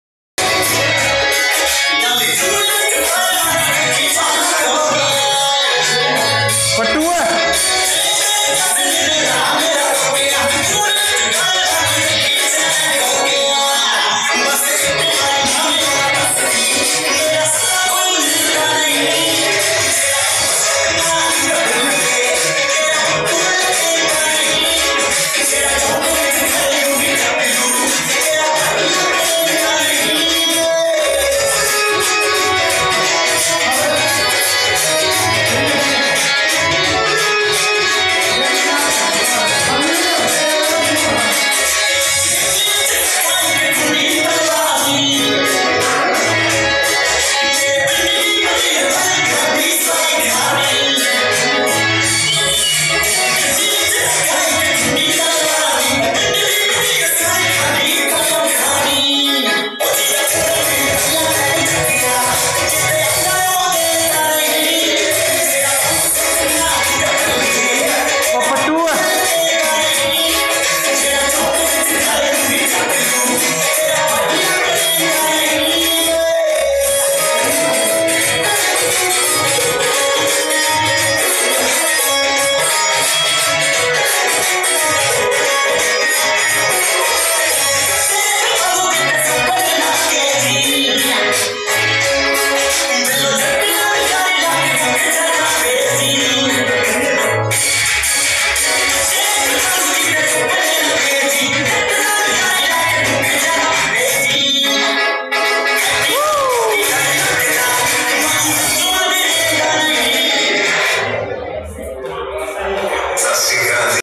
Niilm University Kaithal Breaking the floor Mandeep Banger ft Gaggi paaji
Mandeep malik (pattu) and gagandeep singh (gaggi) amazing dance